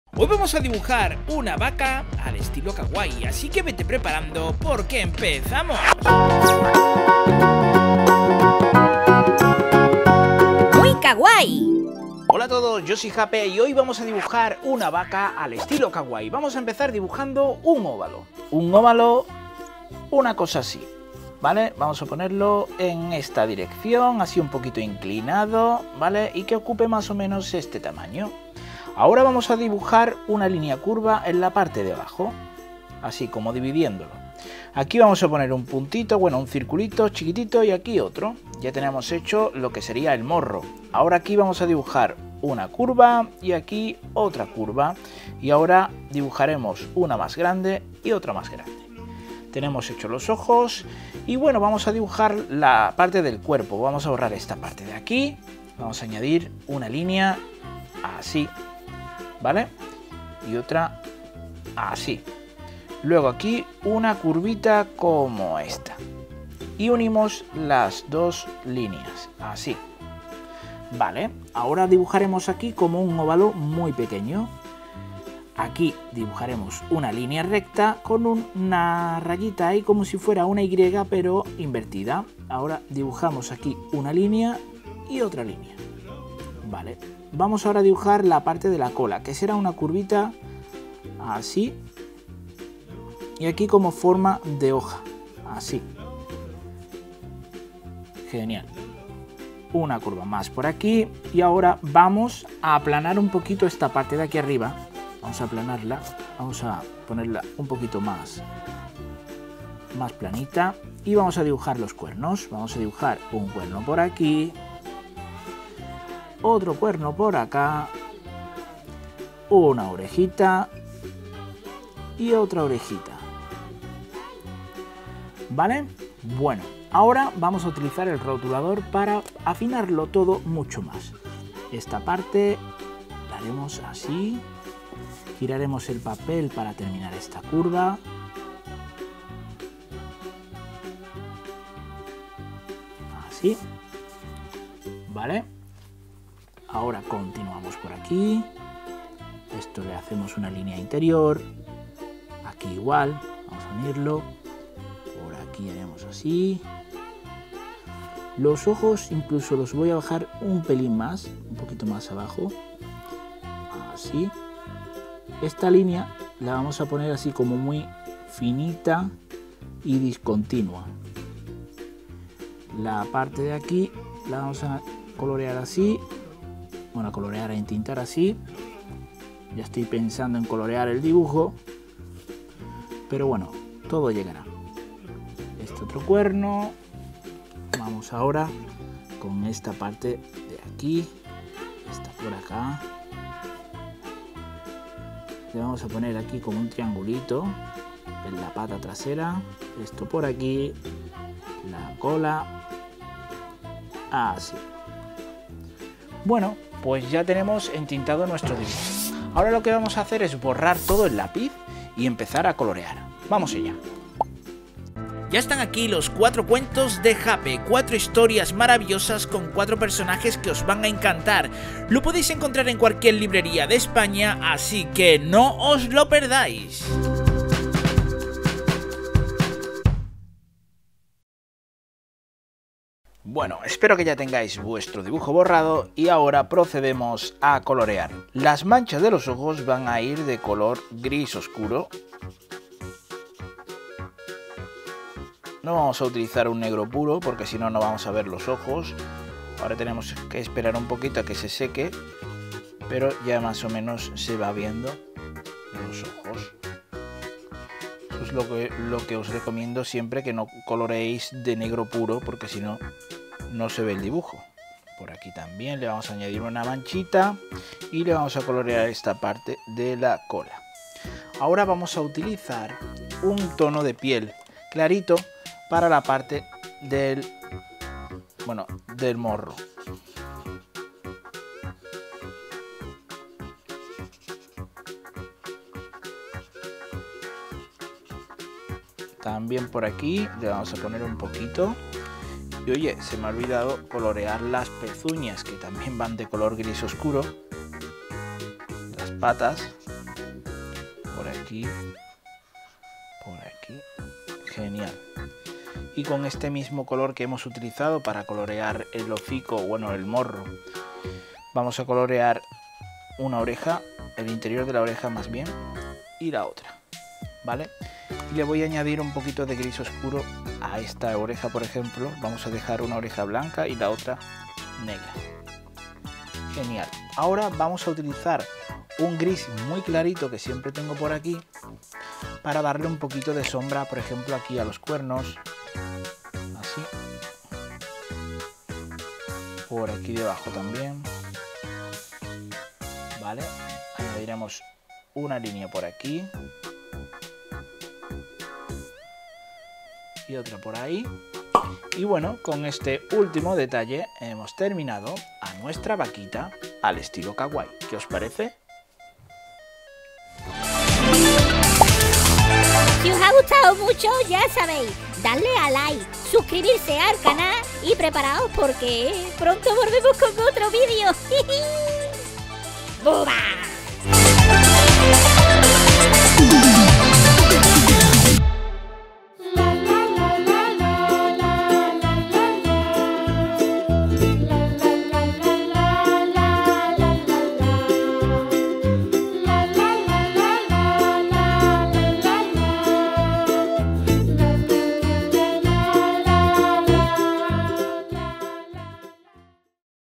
COMO DIBUJAR UNA VACA KAWAII - Dibujos kawaii faciles - Como dibujar animales kawaii
Como dibujar una vaca kawaii es el tema de hoy, aprendera a dibujar mas animales kawaii en el canal.  Y aprende con dibujos kawaii faciles Tengo una serie de como dibujar paso a paso, animales de granja y animales salvajes de todo tipo y de una forma súper fácil.